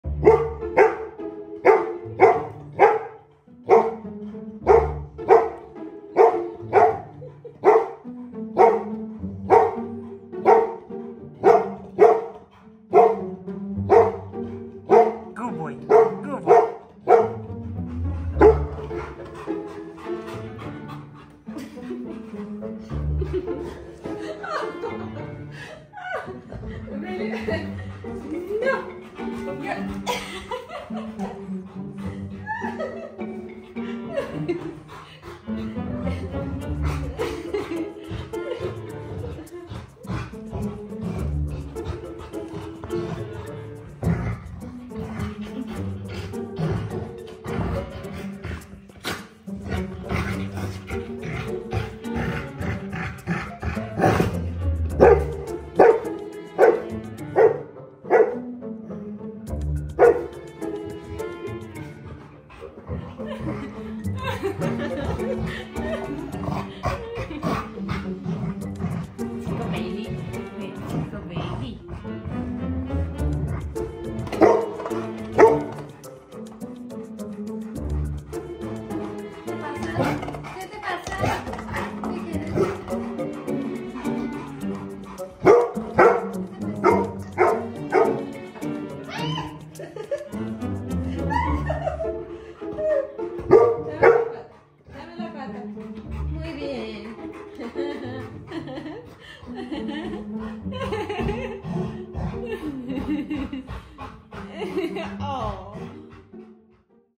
My Golden Retriever Shocked by a Human Mom Occupying his Bed
My Golden Retriever Shocked by a Human Mom Occupying his Bed! 🔔and leave your comments under this video!